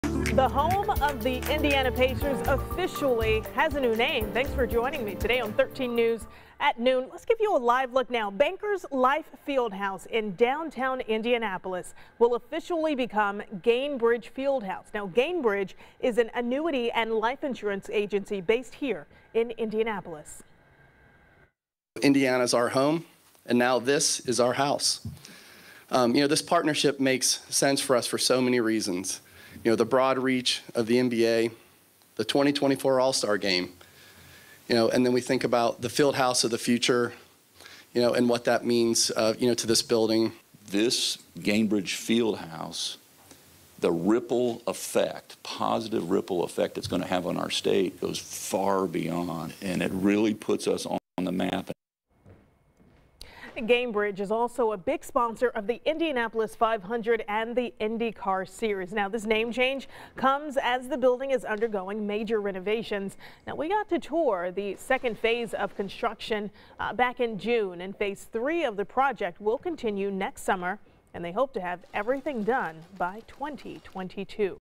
Pacers announce partnership with Gainbridge for naming rights to their fieldhouse
The new name was formally announced at a press conference Monday morning.